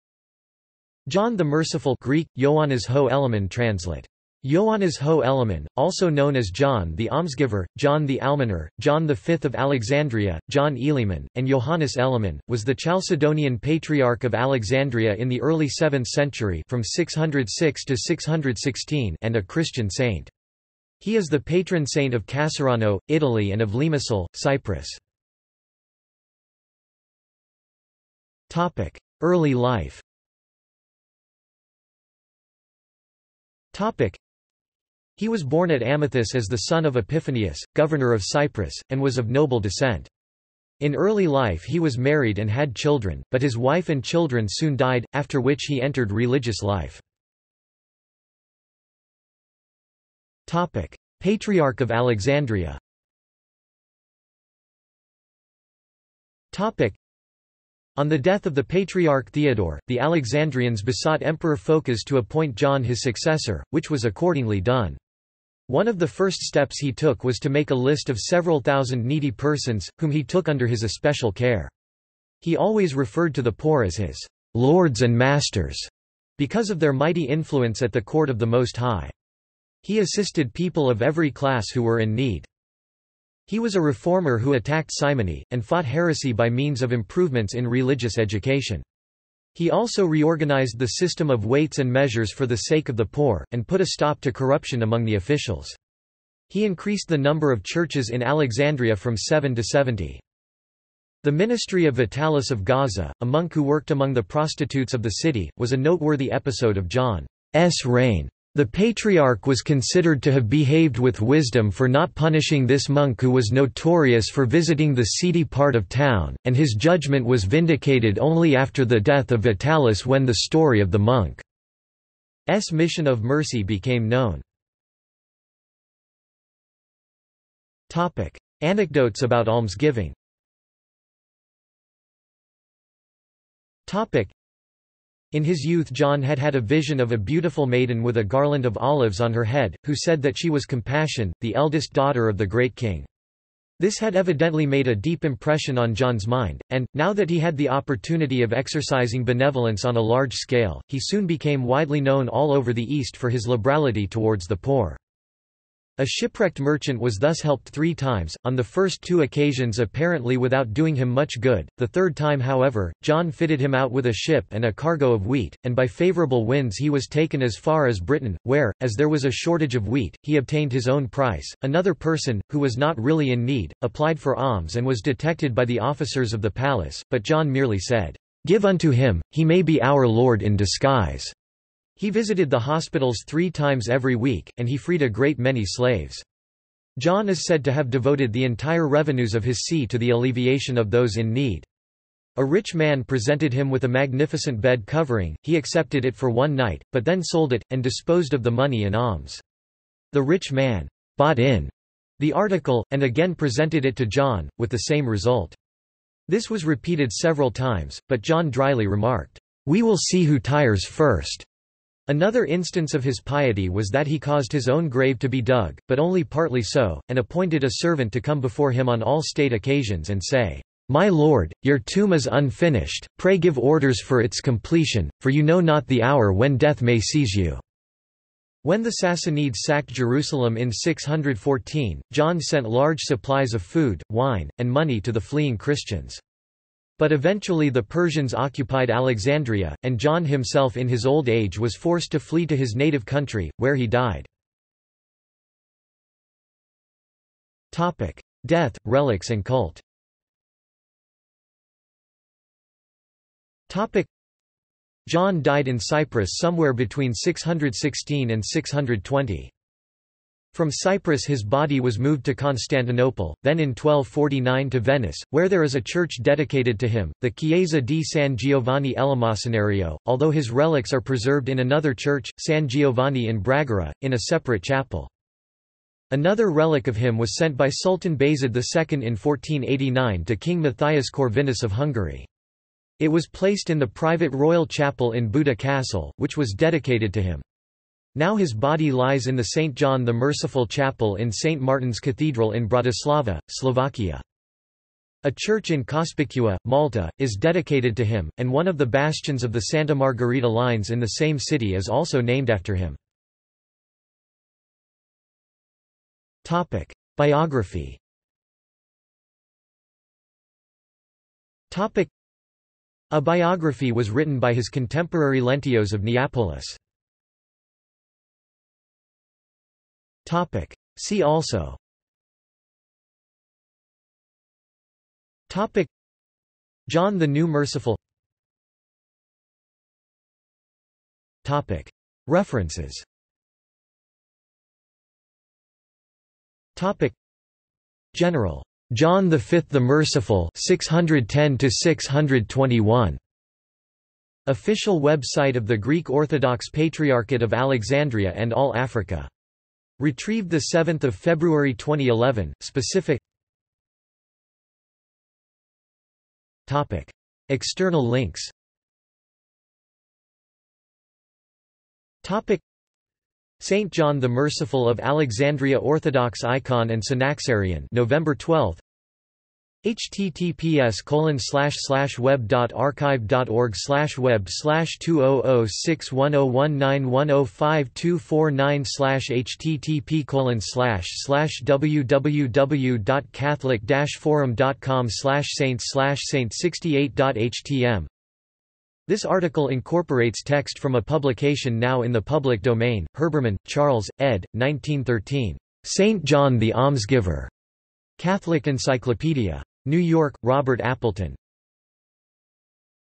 John the Merciful | Wikipedia audio article
This is an audio version of the Wikipedia Article:
John the Merciful

SUMMARY
John the Merciful (Greek: Ἰωάννης ὁ Ἐλεήμων, translit. Iōannēs ho Eleēmōn), also known as John the Almsgiver, John the Almoner, John V of Alexandria, John Eleymon, and Johannes Eleemon, was the Chalcedonian Patriarch of Alexandria in the early 7th century (from 606 to 616) and a Christian saint. He is the patron saint of Casarano, Italy and of Limassol, Cyprus.